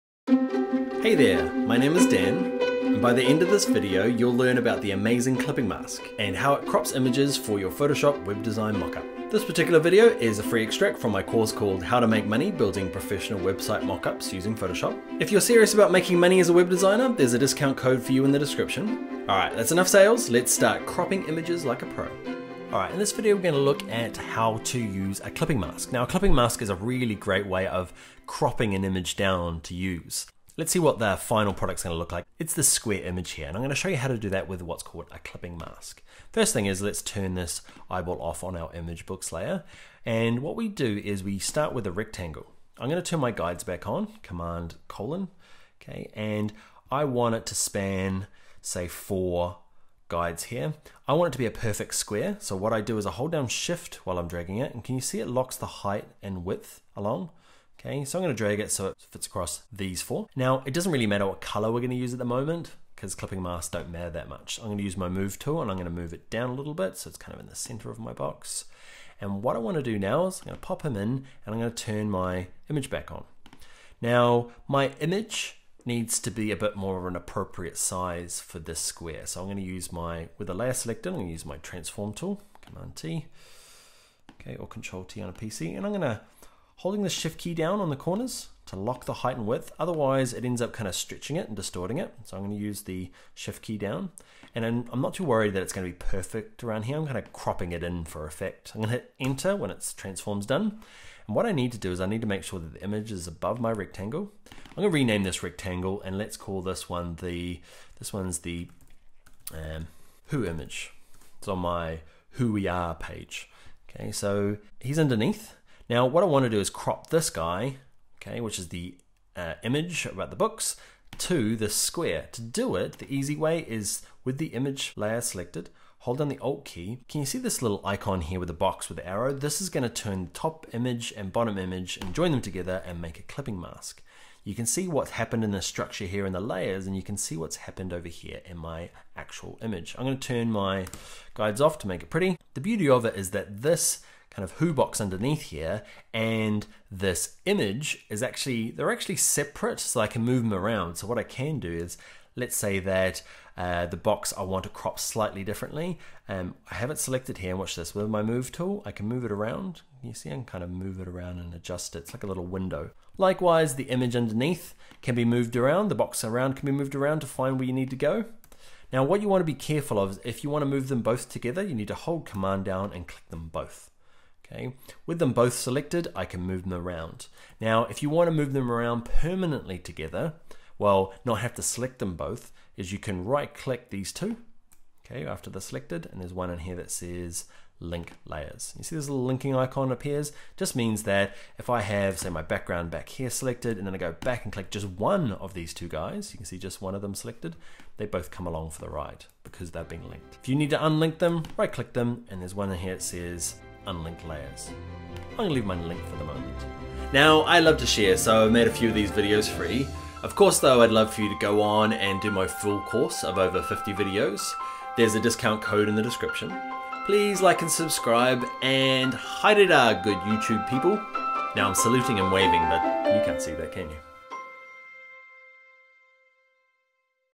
Photoshop CC 2015 Web Design Tutorial | Clipping Mask in Photoshop for Websites - 31/48
Photoshop CC 2015 Web Design Tutorial, How to make a Clipping Mask in Photoshop for Websites 31/48

Video transcript:

0:00hey there my name is Dan by the end of
0:03this video you'll learn about the
0:04amazing clipping mask and how it crops
0:06images for your Photoshop web design
0:08mockup this particular video is a free
0:10extract from my course called how to
0:12make money building professional website
0:13mock-ups using Photoshop if you're
0:15serious about making money as a web
0:16designer there's a discount code for you
0:18in the description all right that's
0:19enough sales let's start cropping images
0:21like a pro alright in this video we're
0:23going to look at how to use a clipping
0:25mask now a clipping mask is a really
0:27great way of cropping an image down to
0:29use let's see what that final products
0:31going to look like it's the square image
0:33here i'm going to show you how to do
0:34that with what's called a clipping mask
0:35first thing is let's turn this i bought
0:38off on our image books layer and what we
0:40do is we start with a rectangle i'm
0:42going to turn my guides back on command
0:44colon okay and I want it to span say for
0:49guides here I want it to be a perfect
0:51square so what I do is a hold down shift
0:53while I'm dragging it and can you see it
0:55locks the height and width alone okay so
0:57I'm going to drag it so it fits across
0:59these for now it doesn't really matter
1:01what color we're going to use at the
1:02moment because clipping masks don't
1:03matter that much I'm going to use my
1:04move tool and I'm going to move it down
1:06a little bit so it's kind of in the
1:07center of my box and what I want to do
1:09now is pop them in and I'm going to turn
1:11my image back on now my image needs to
1:16be a bit more of an appropriate size for
1:18this square so I'm going to use my with
1:20a layer select and use my transform tool
1:22come on T okay or ctrl key on a PC and
1:25I'm going to holding the shift key down
1:27on the corners to lock the height and
1:29width otherwise it ends up kind of
1:30stretching it and distorting it so I'm
1:32going to use the shift key down and then
1:34I'm not too worried that it's going to
1:35be perfect around here I'm kind of
1:37cropping it in for effect and hit enter
1:39when it's transforms done what I need to
1:41do is I need to make sure that the image
1:43is above my rectangle i'm going to
1:44rename this rectangle and let's call
1:46this one the this one's the and who
1:50image so my who we are page okay so he's
1:54underneath now what I want to do is crop
1:56this guy okay which is the image read
2:00the books to the square to do it the
2:03easy way is with the image layer
2:05selected hold down the alt key can you
2:07see this little icon here with the box
2:08with arrow this is going to turn top
2:10image and bottom image and join them
2:12together and
2:13make a clipping mask you can see what
2:15happened in the structure here in the
2:16layers and you can see what's happened
2:17over here in my actual image I don't
2:20turn my guides off to make it pretty the
2:22beauty of it is that this kind of who
2:24box underneath here and this image is
2:28actually they're actually separate so I
2:30can move them around so what i can do is
2:31let's say that the box i want to crop
2:34slightly differently and i haven't
2:37selected here watch this will my move
2:38tool I can move it around you seem kind
2:40of move it around and adjust it's like a
2:42little window likewise the image
2:44underneath can be moved around the box
2:46around can be moved around to find where
2:47you need to go now what you want to be
2:49careful of if you want to move them both
2:51together you need to hold command down
2:52and click them both okay with them both
2:55selected I can move them around now if
2:57you want to move them around permanently
2:59together well not have to select them
3:02both is you can right-click these two
3:04okay after the selected and there's one
3:06in here that says link layers you see
3:08this linking icon appears just means

Trending topic on youtube